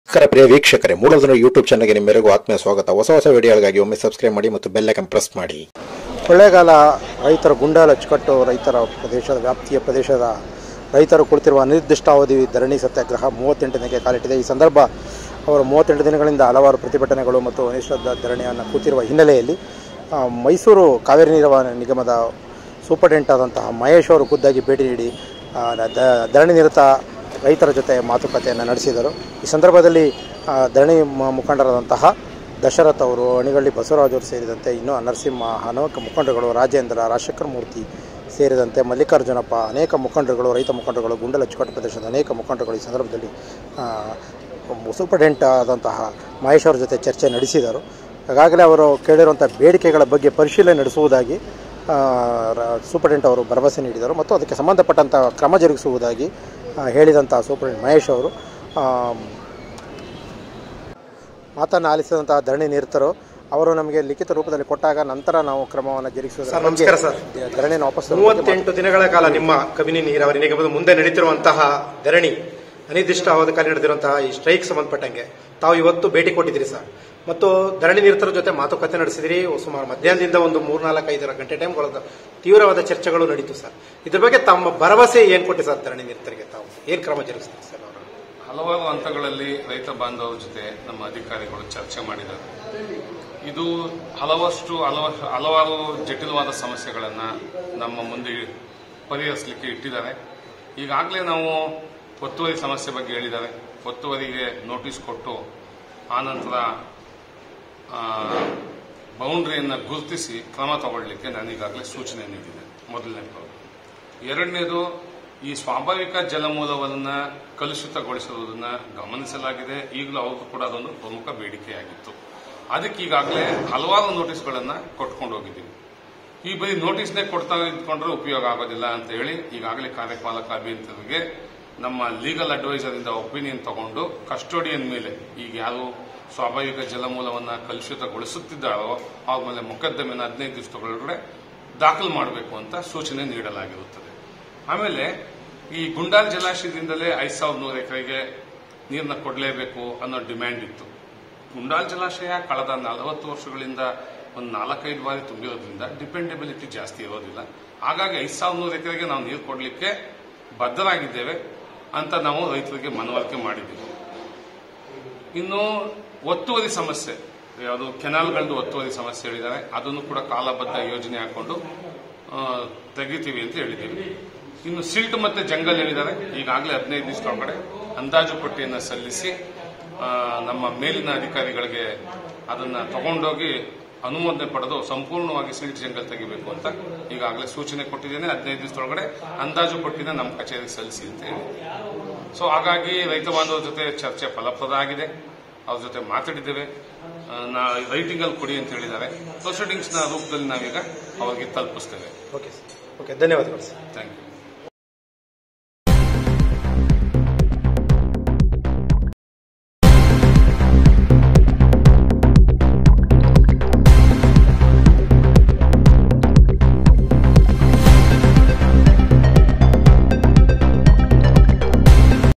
ಕಾವೇರಿ ನೀರಾವರಿನಿಗಮ se ಮಹೇಶ್ ಹೇಳಿದ್ದೇನು ಕೊಳ್ಳೇಗಾಲನೀರಾವರಿ ಇಲಾಖೆ ರೈತರ ಧರಣಿ 38 ದಿನ ; ಬೇಡಿಕೆ ಪರಿಶೀಲನೆ
ಮೂಡಲ ಧ್ವನಿ ಕನ್ನಡ ನ್ಯೂಸ್ ‌ಯುಟ್ಯೂಬ್ ಚಾನೆಲ್ ಗೆ ಸಬ್ ಸ್ಕ್ರೈಬ್ ಮಾಡಿ ಬೆಲ್ ಐಕಾನ್ ಪ್ರೆಸ್ ಮಾಡಿ ಶೇರ್ ಮಾಡಿ ಲೈಕ್ ಮಾಡಿ ಕಮೆಂಟ್ ಬಾಕ್ಸಲ್ಲಿ ಕಮೆಂಟ್ ಮಾಡಿ